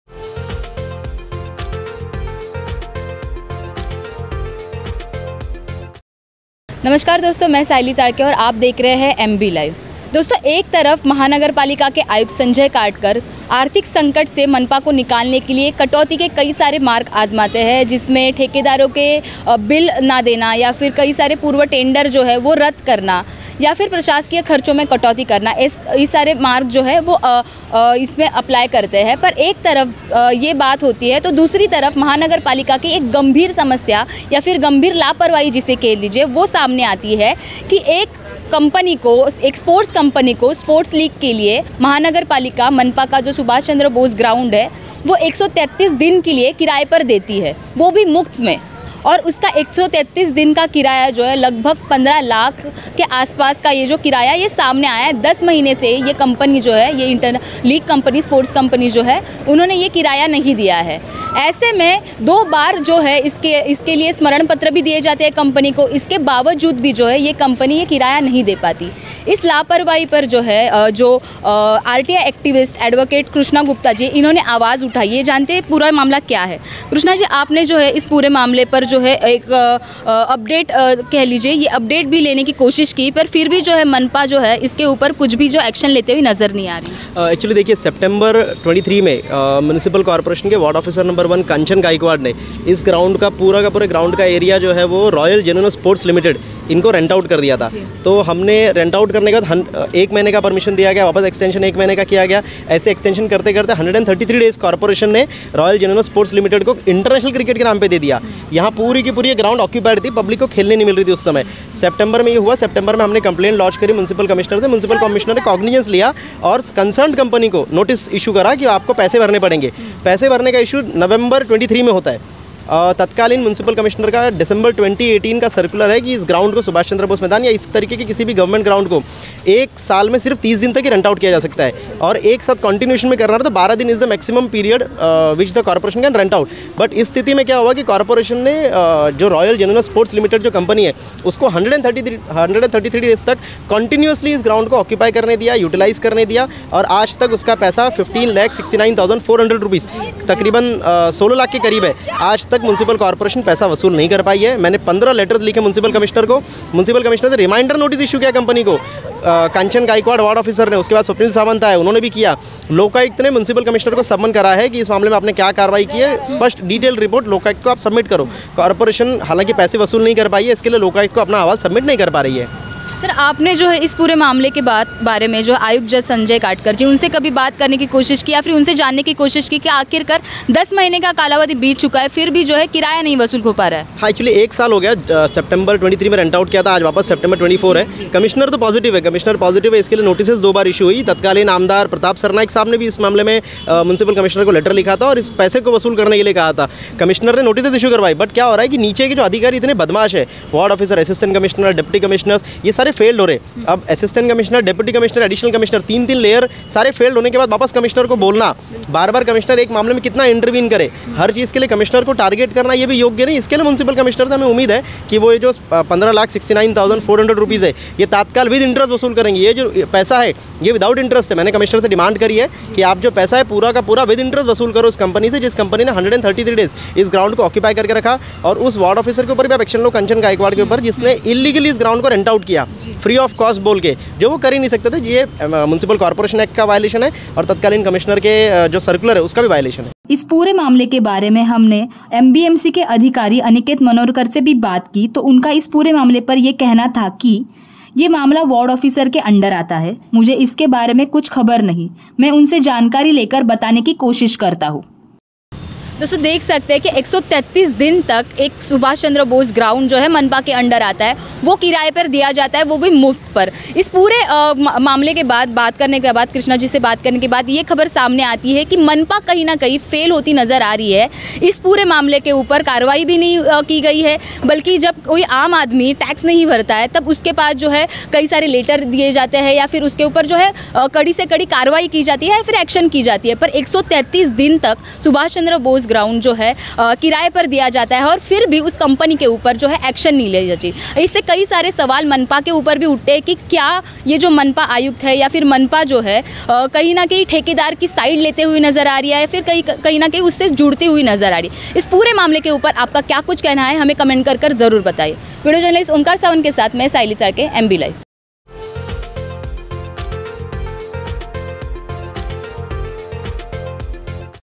Krishna Gupta ने फिर किया बड़ा खुलासा, MBMC का एक और घोटाला उजागर! | Mira Bhayandar | MB Live Hindi
It's been ten months, and the sports company still hasn't paid the dues! | Despite multiple reminders from MBMC, the company continues to ignore the payment requests, raising serious questions about possible collusion within MBMC | Why is MBMC struggling to collect the rent, and what are the implications of this ongoing issue?